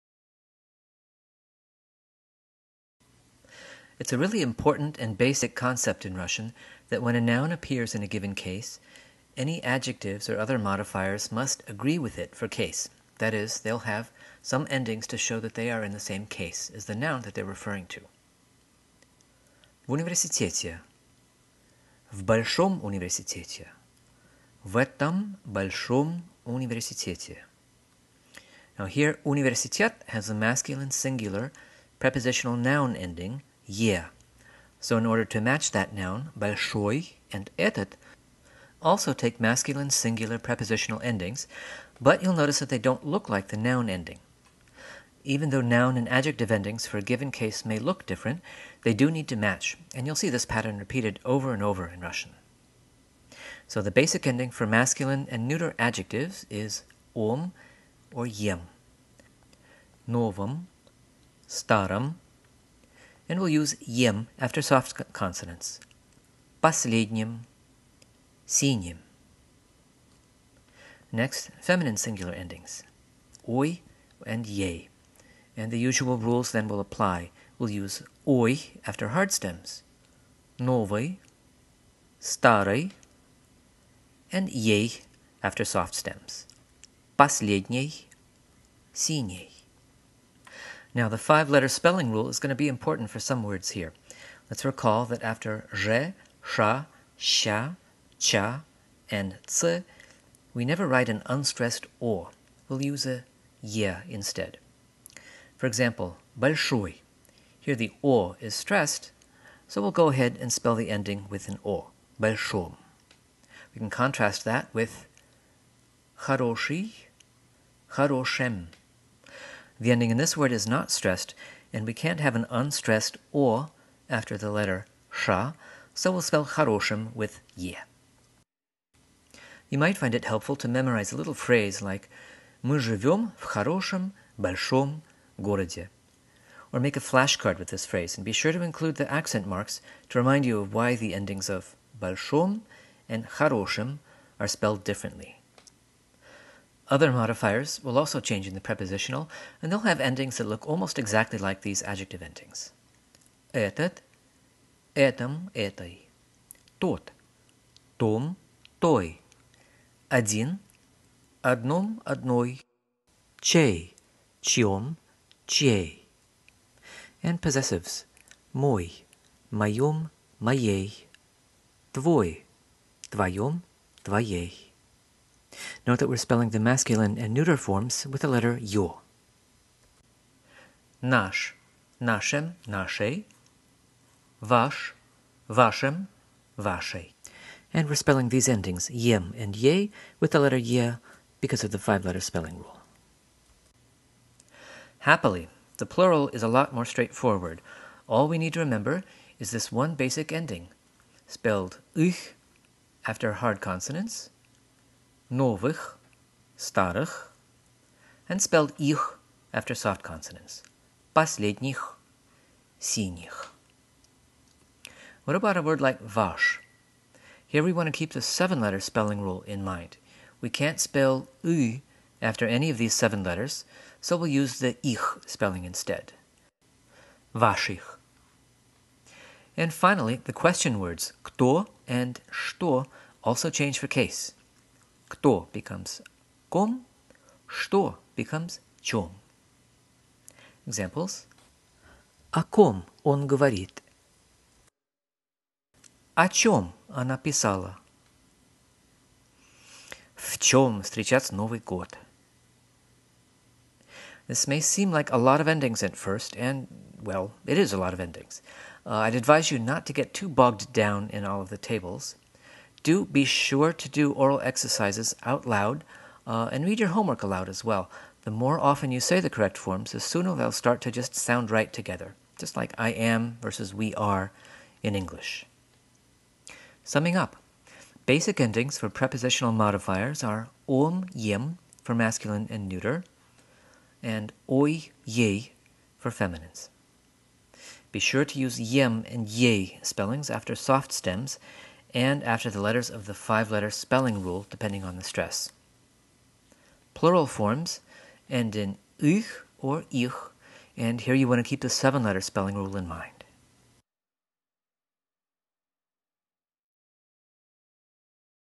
Russian Cases: The Prepositional Case of Modifiers
For beginners: an overview of the prepositional case forms of modifiers (adjectives, etc) in Russian. You'll use these forms to describe a location, or something you're talking 'about': "in a big city," "in a small apartment," or "about my books."

To get the most of this tutorial, you should be comfortable with concepts of hard and soft consonants.
NOTE: for consistency, at 1:2, the two last forms at the left should be последняя and синяя (feminine).

For over 200 videos like this one, and over 400 embedded comprehension checks, exercises, quizzes, and example sentences, visit my Russian Grammar Library Project